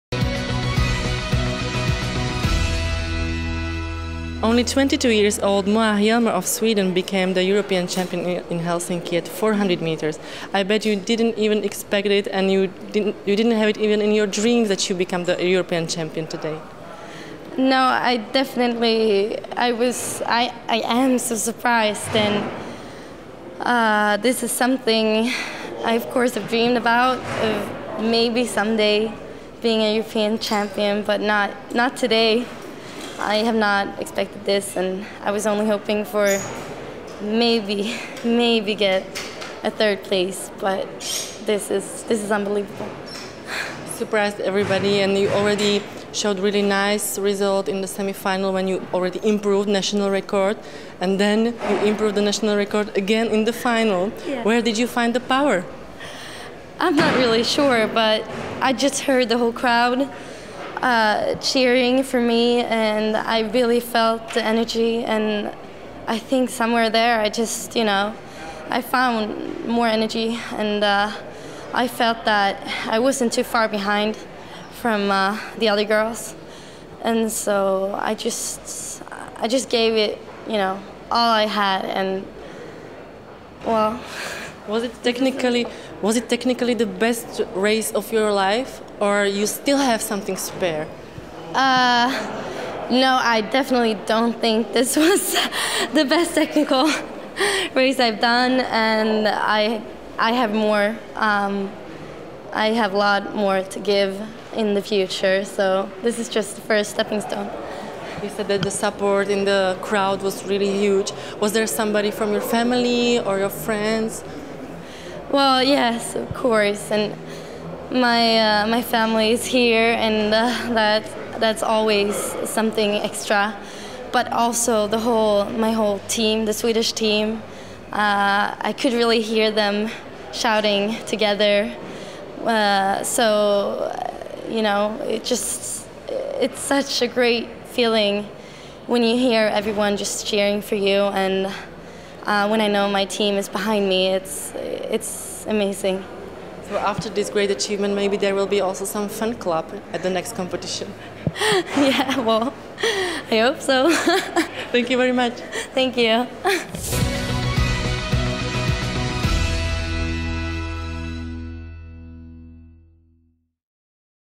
ECH2012 Helsinki Day 3 Moa HJELMER (SWE)
Moa Hjelmer speaks to European Athletics after 400m Women Final